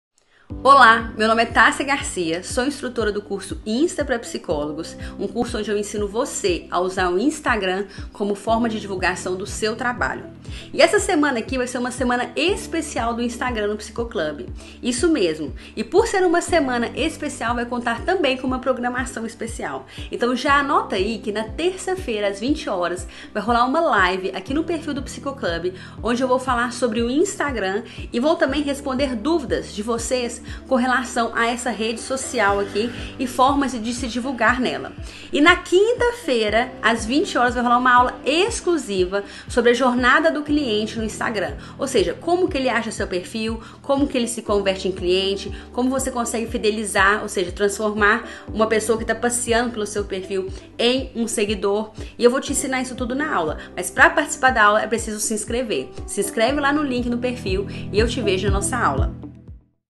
Jornada do Cliente no Insta - Convite
Você vê em diversos lugares que ter um instagram é o futuro do seu negócio, sendo ele online ou não. Mas como alguém que te segue ou curte o seu perfil torna-se um cliente? É possível identificar pessoas que irão pagar pelo seu serviço sem ficar chato ou deselegante?

Para entender melhor esse mundo novo, participe dessa aula com a Tassia Garcia e entenda como o instagram pode fazer a diferença no seu trabalho como psicólogo.

A aula acontece nesta quinta-feira 19/09 às 20h, e para participar gratuitamente é só se inscrever aqui: